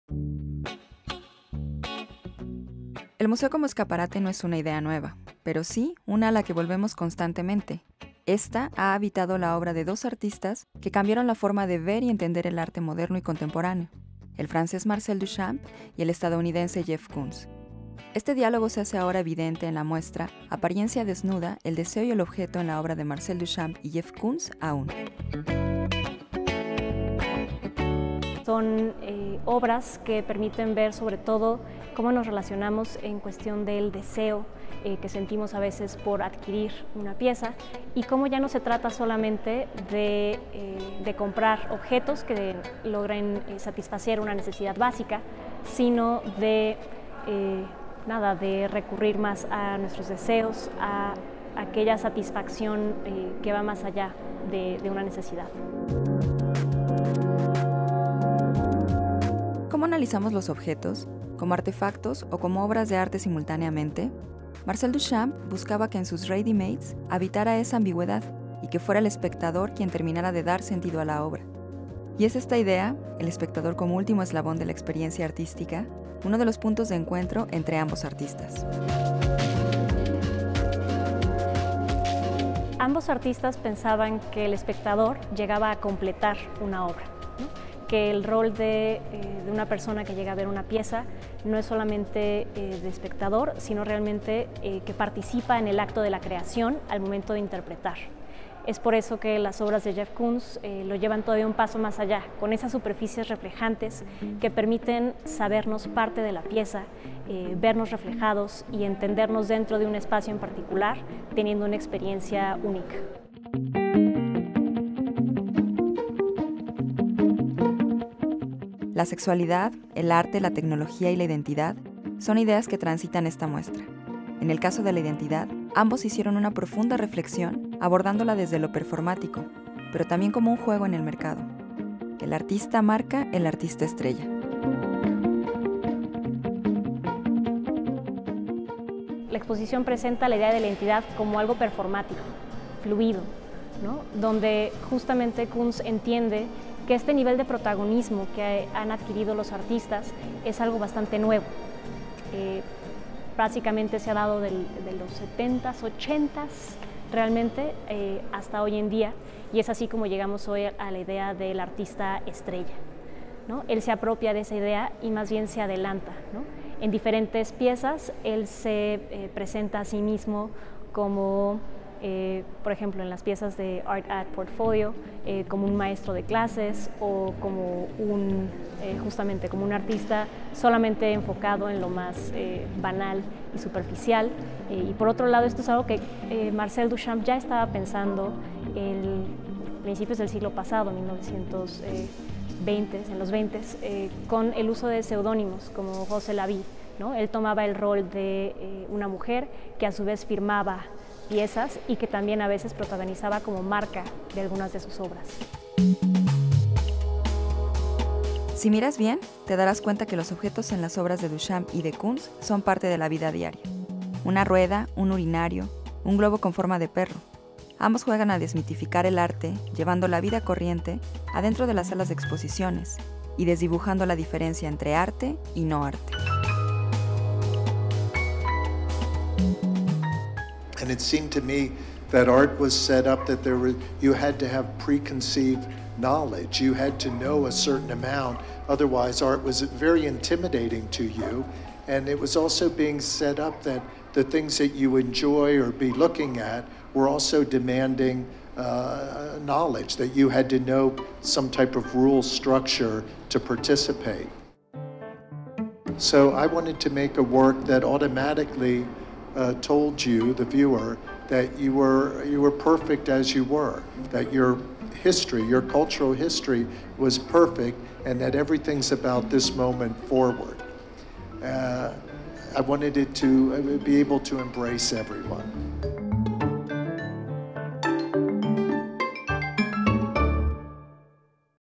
Koons y Duchamp juntos en la exposición "Apariencia desnuda".
La obra de dos exponentes del arte moderno y contemporáneo en una misma sede, el Museo Jumex.
¿Cuál es el eslabón que une el pensamiento en torno al arte de ambos artistas? Acompáñanos a descubrirlo en una charla con la asistente curatorial de la exposición, María Emilia Fernández.